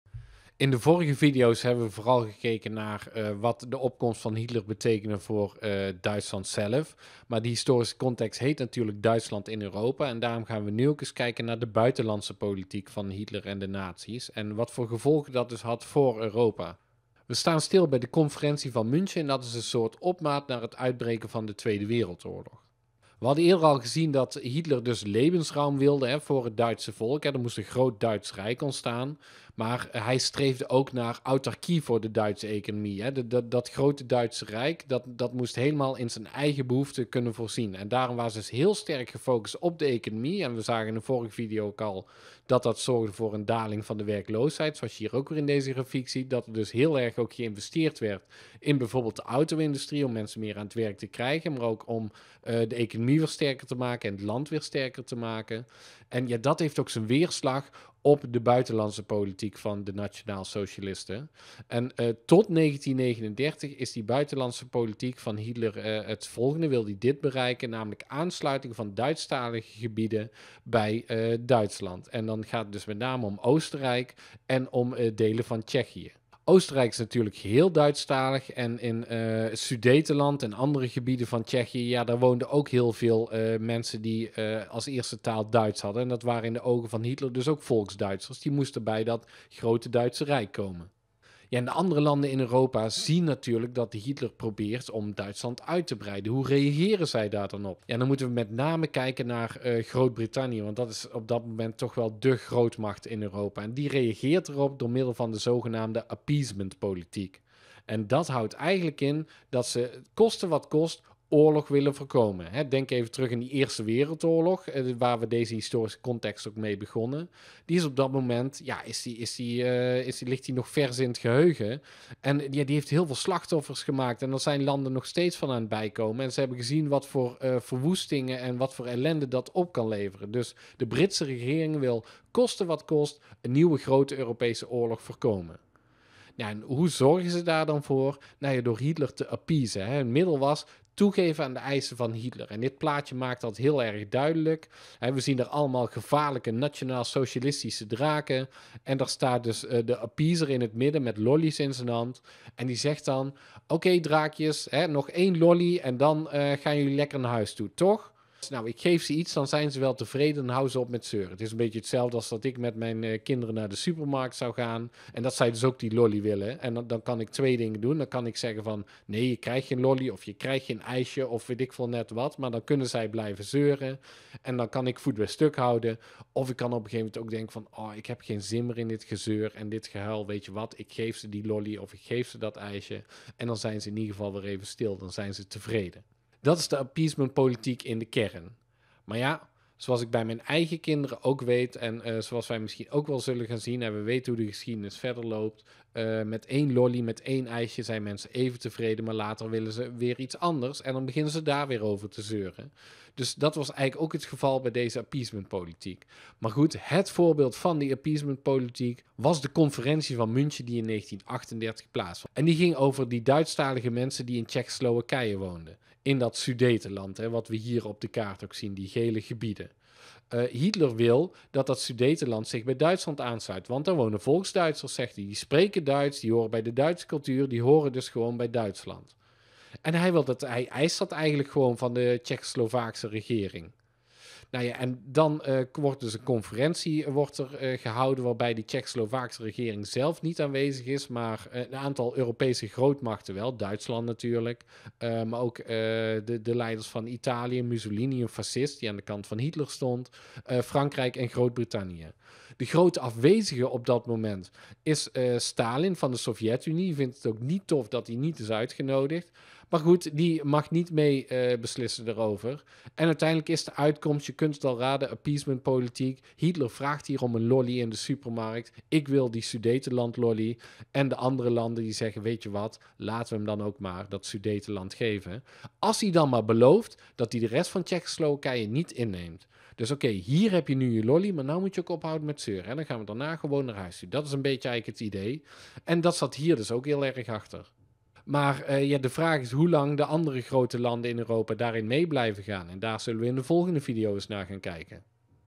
8. De conferentie van München (HAVO/VWO HC Duitsland in Europa)
Deze video is voor de bovenbouw van de havo en vwo. Hij hoort bij de historische context ‘Duitsland in Europa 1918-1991'.

Uit de stofomschrijving:
De eis tot aansluiting van Duitstalige gebieden leidde tot de Conferentie van München in
1938. Met de appeasementpolitiek probeerde Groot-Brittannië oorlog nog te voorkomen.

Afbeeldingen:
- eigen collectie